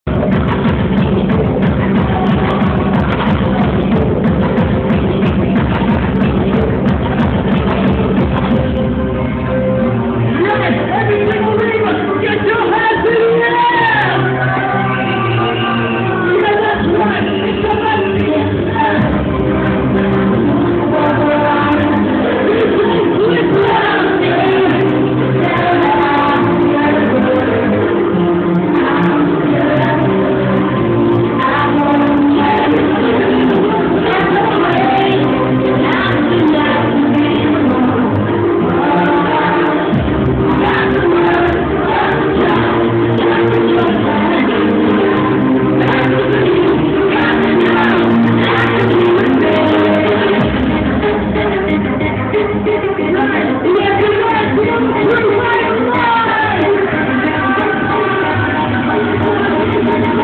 Vibealite 02/03/07
just a quick clip of Vibealites main room. the crowd were singing their hearts out!!! lol